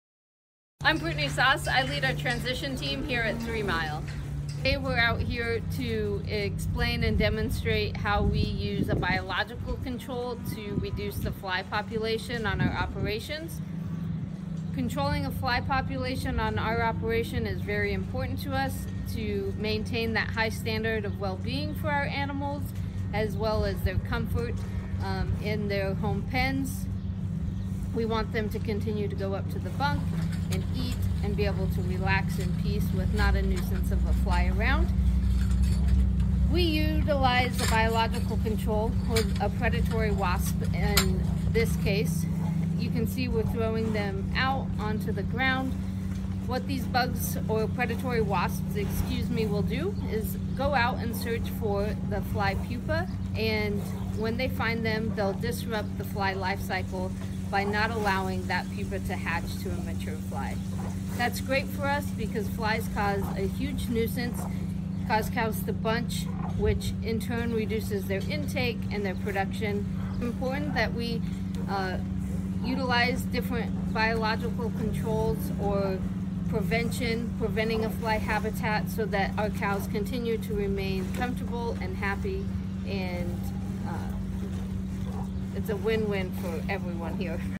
Talking Integrated Pest Management at Threemile
Keeping our cows calm and stress-free is a top priority for our team. Fly pressure can reduce daily milk production on our farm by over 8,000 gallons. One part of our integrated pest management plan is utilizing a biological control, in this case, predatory wasps. How do these predatory wasps play a crucial role in maintaining an optimal environment for our cattle? Threemile Maternity Manager, Brittany Szasz explains!